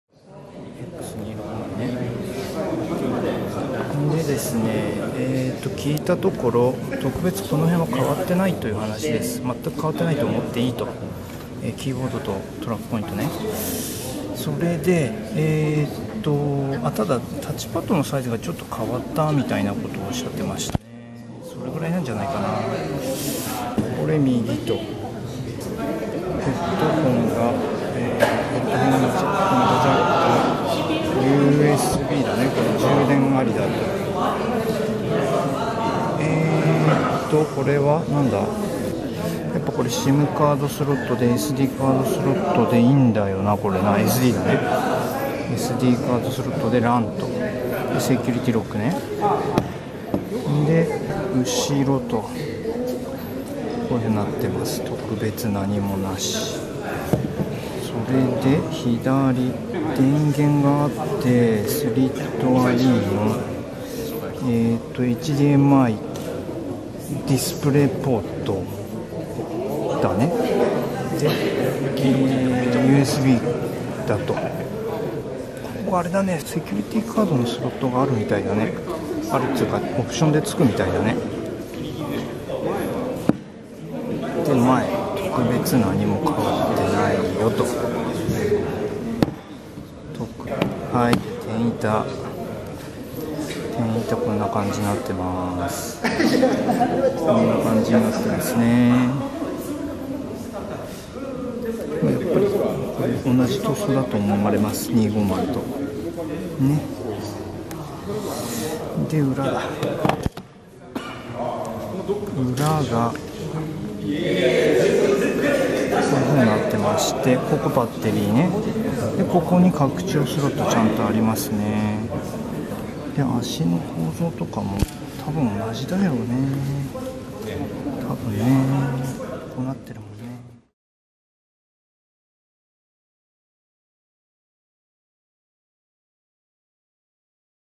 Lenovo ThinkPad X260の外観ﾁｪｯｸ!![X250後継,ｼﾝｸﾊﾟｯﾄﾞ]
2016/01/19秋葉原で行われた「Thinkシリーズ新製品記者発表会」より。

ThinkPad X260の外観チェックです。
X250と比べると、
・キーボードとトラックポイントはX250と全く同じと思って間違いない。細かい事を言うと、タッチパッドのサイズが若干変わったらしい。実際使ってみた感じでは、X250と全く変わらないと思いました。
・VGAが無くなった
・USBポートが一つ増えた(3つに)
・ディスプレイ出力はHDMIとmini DisplayPortに